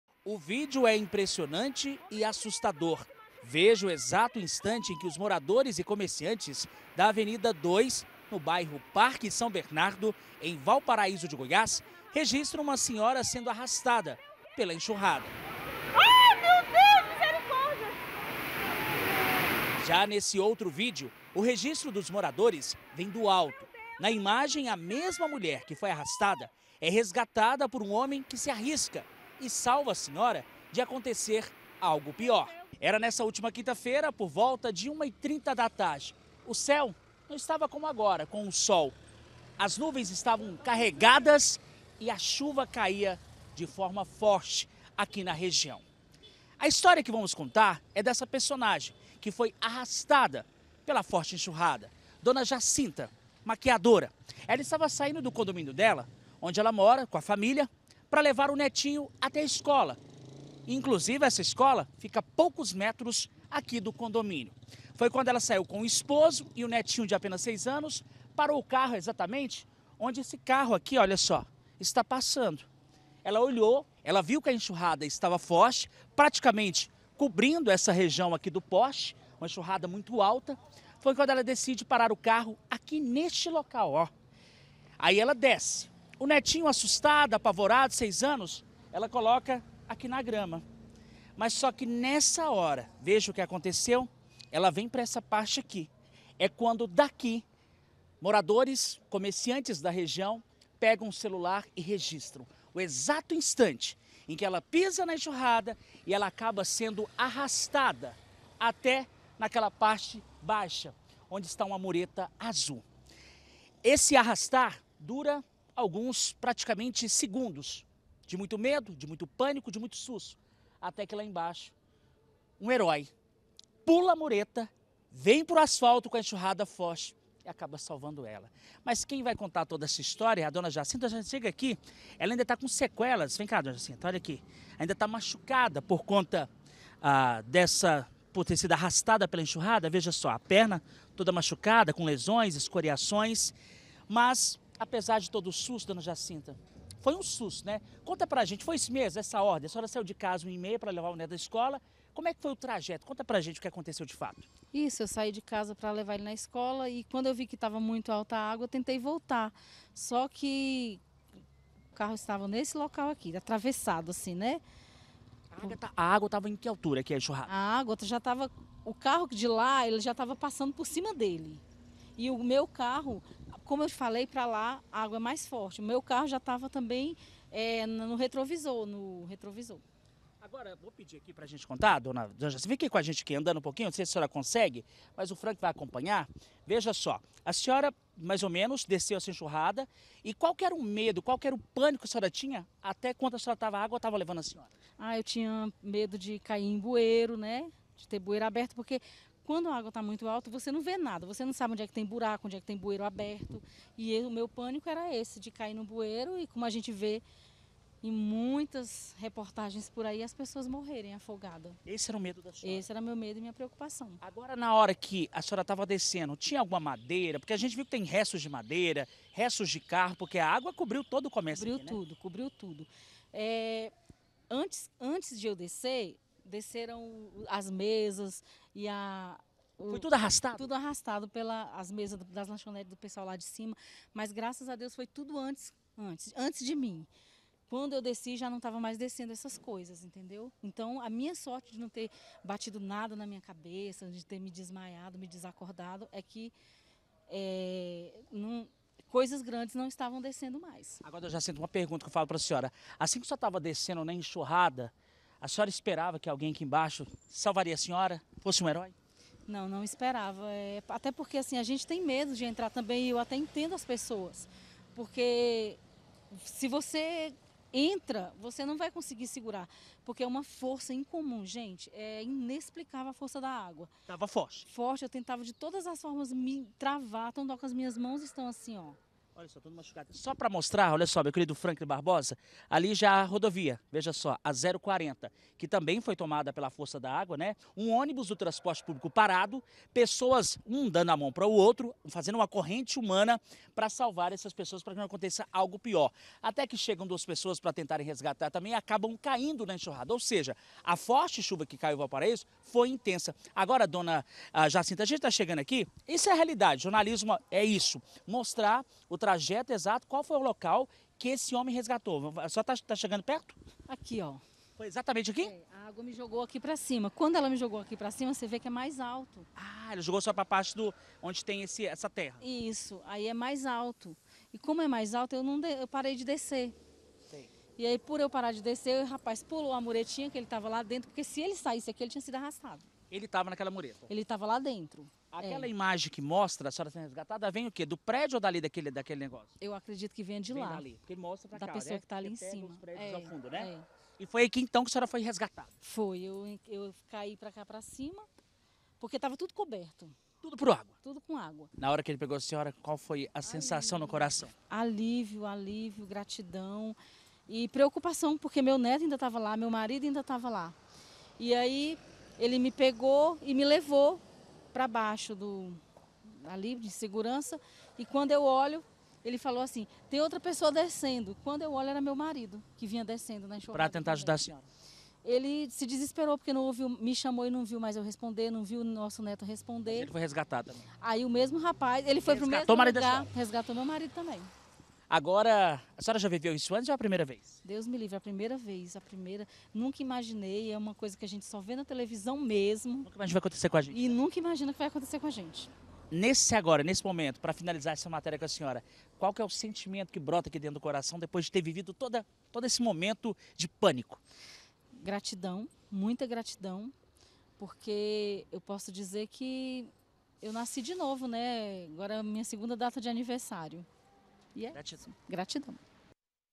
ARRASTADA PELA ENXURRADA: DONA JACINTA CONTA COMO ESCAPOU PRATICAMENTE ILESA
MOMENTOS DE ANGÚSTIA E DESESPERO. UMA MULHER SENDO LEVADA PELA FORÇA DA ENXURRADA. APESAR DO SUSTO, NADA DE MAIS GRAVE ACONTECEU COM ELA. ESSA HISTÓRIA IMPRESSIONANTE VOCÊ ACOMPANHA AGORA, NA REPORTAGEM DE PAULO VITOR TATUZINHO.